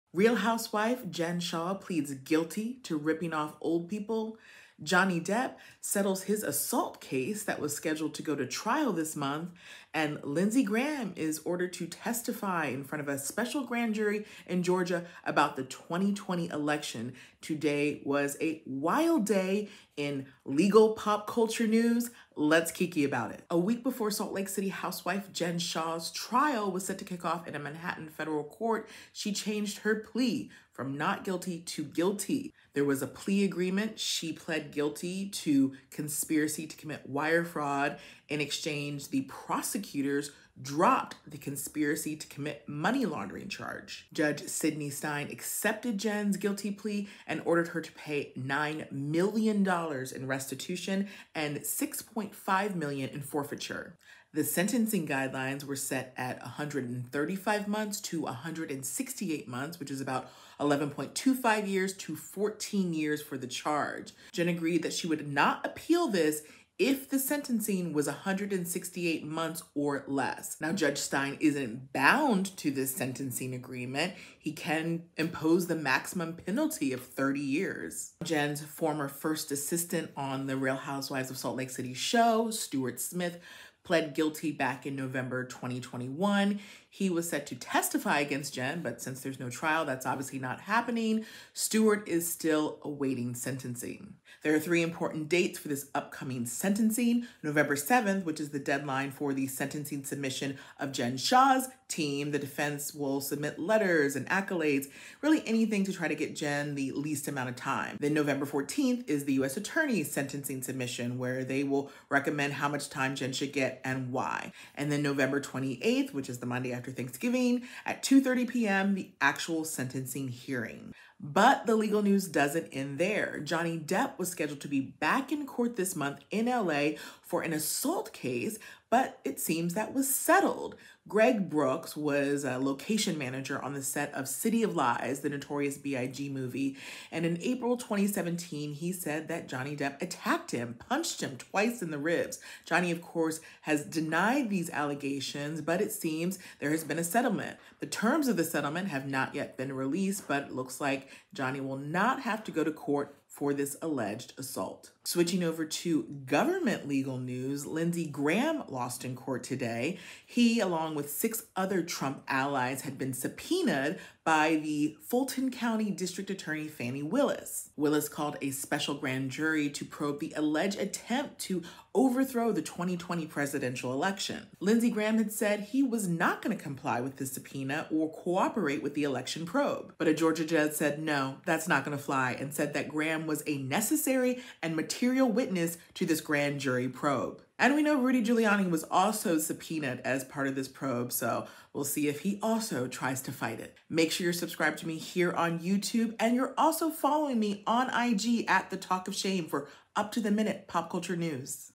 Today's Pop Culture Legal Wrap Up - Jen Shah guilty, Johnny Depp avoids trial, Lindsey Graham loses
Today was a wild day in pop culture legal news,  Real Housewife of Salt Lake City, Jen Shah, changed her plea from not guilty to guilty and here's what she owes, how much time she may do, and when she will be sentenced.

Johnny Depp was sued for assault by a crewmember in 2018 and was set to go to finally go court to court this month, but a notice of settlement was filed.

Lindsey Graham was subpoenaed to appear before a Georgia special grand jury to answer questions about Trump attempt to overturn the 2020 election and Graham vowed not to comply, but a judge says he is a "necessary and material" witness.